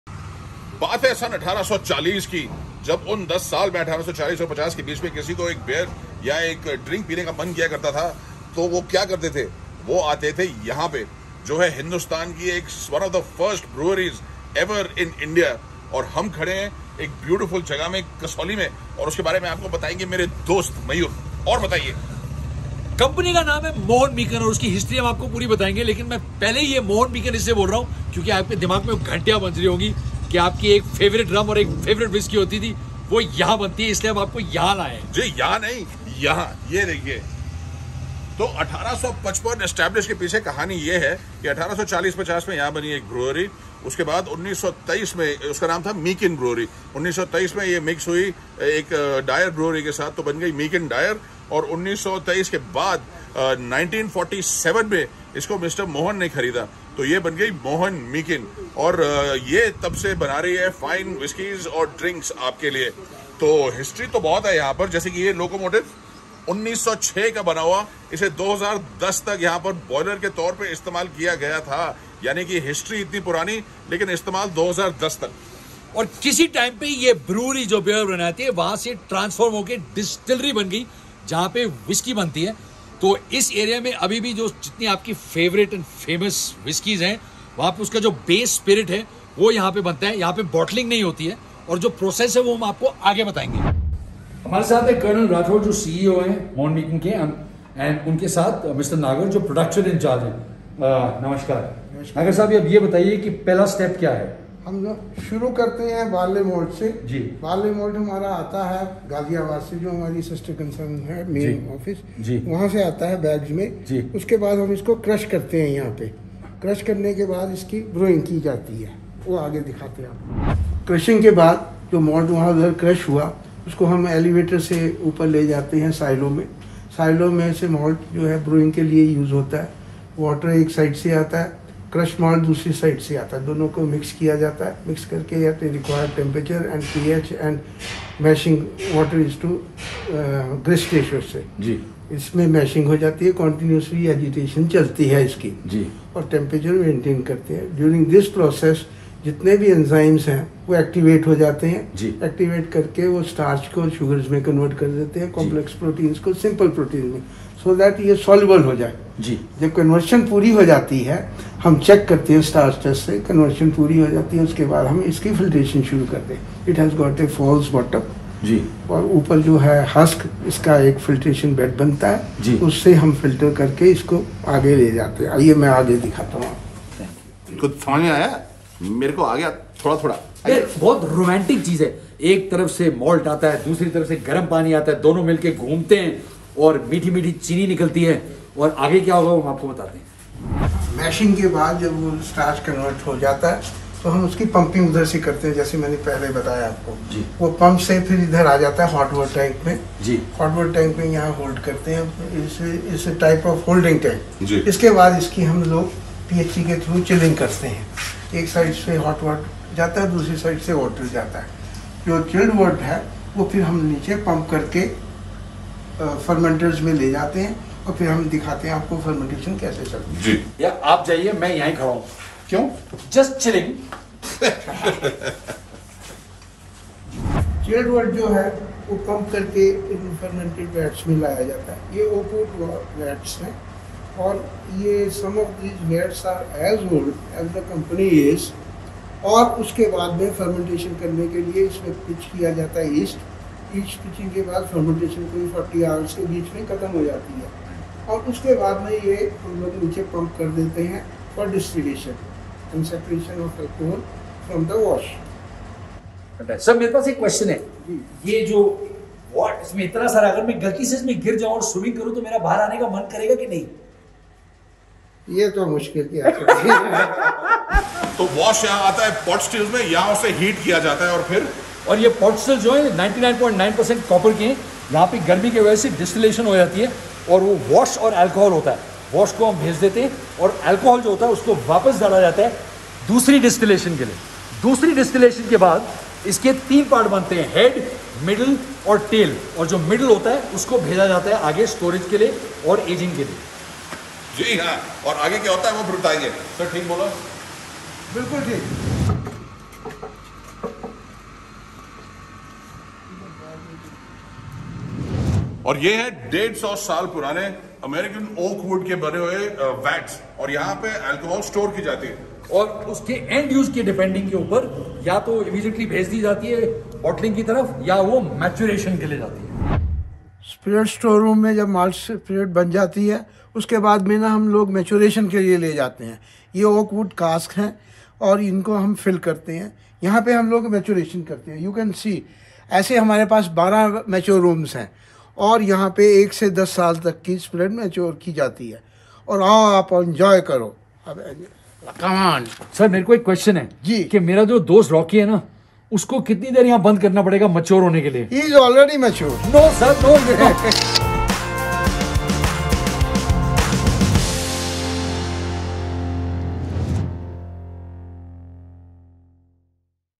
RoadTrippinwithRnM S2 | Day 2 | Vlog 05 | Rocky Mayur | Kasauli Brewery
देखिए ग्लास तक पहुँचने से पहले किन-किन रास्तों से गुजरना पड़ता है व्हिसकी को! RoadTrippinwithRnM S2

How interesting! Step by step how whiskey is made in India. Learn from the master and remember to be mature 😋 RoadTrippinwithRnM S2